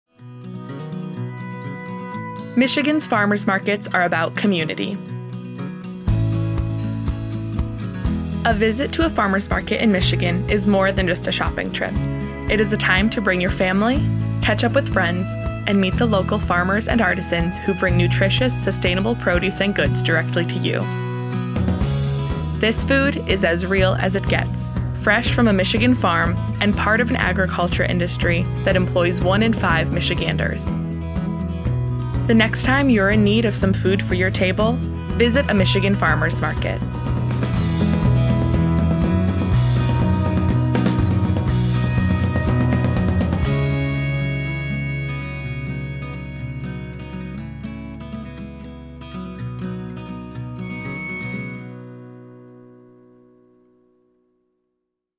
Michigan Farmers Markets | MEDC
Beyond the $104.7 billion that food and agriculture contributes to our economy, employing 1 out of every 5 people in Michigan, and from leading the nation in the production of asparagus, black beans, tart cherries, cucumbers, squash, and several other commodities, there is pride embedded in everything grown and sold in Michigan.